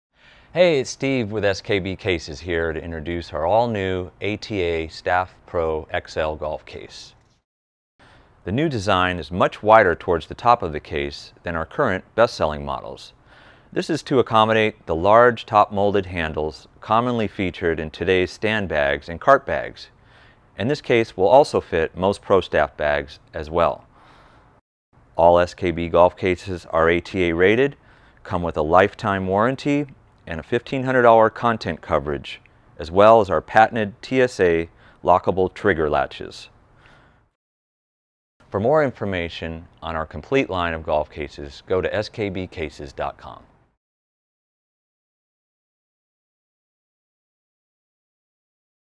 ATA Staff Pro XL Golf Travel Case
SKB's new ATA Staff Pro XL Golf Travel Case (2SKB-SPXL) was developed with a wider diameter throughout the body and neck to fit today's popular cart and stand bags that feature molded, protruding top handles. Here's the VP of the Sports Division with more info on this case and our complete lineup of world-class golf travel cases.